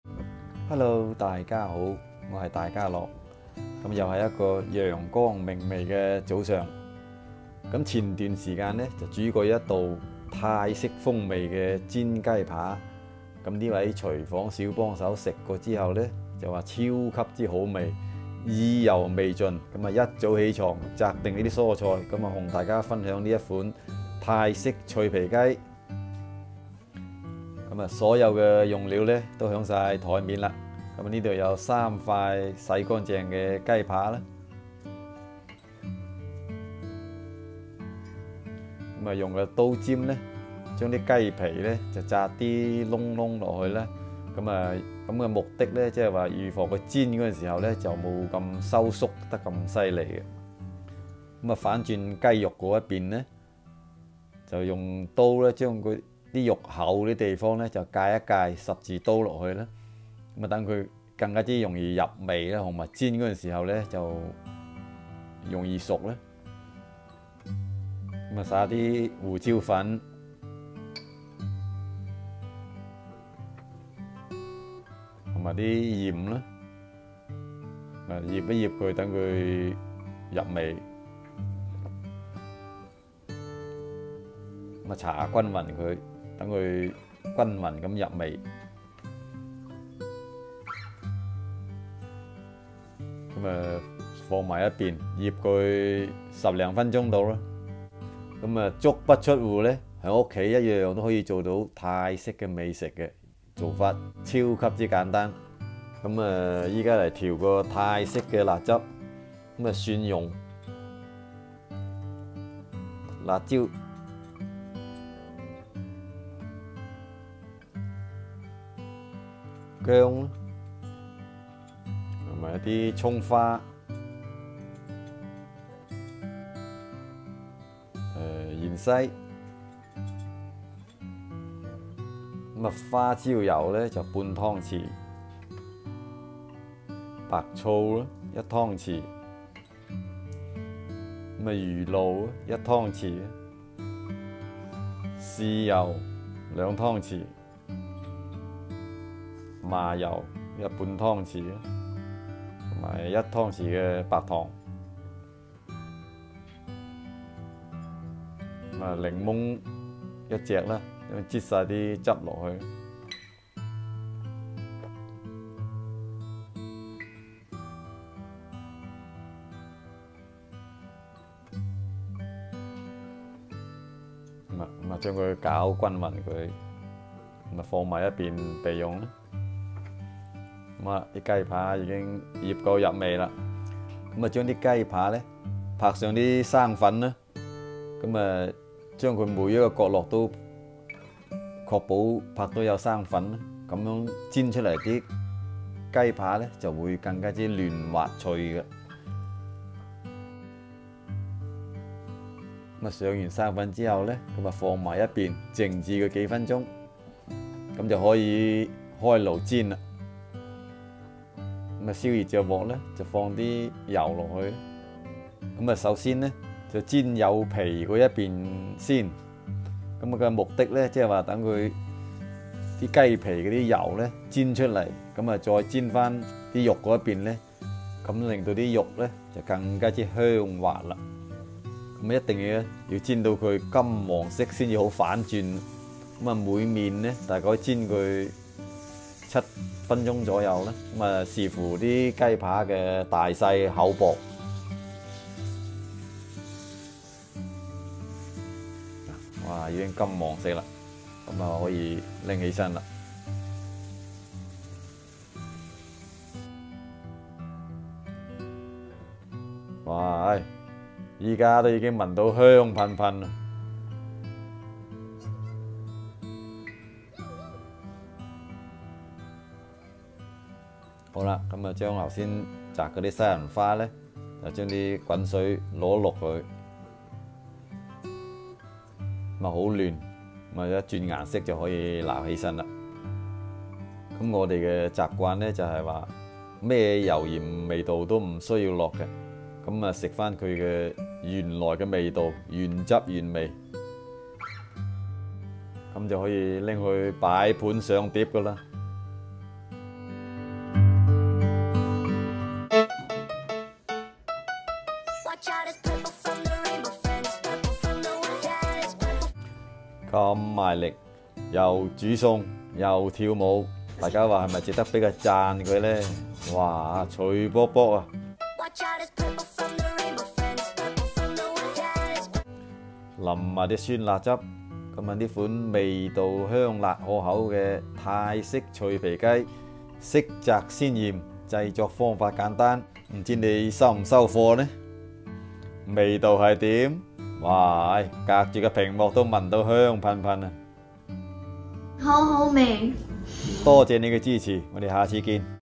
泰式脆皮鸡，鸡扒千万不要直接下锅煎，这样做出来才会皮脆肉嫩！
美食分亨，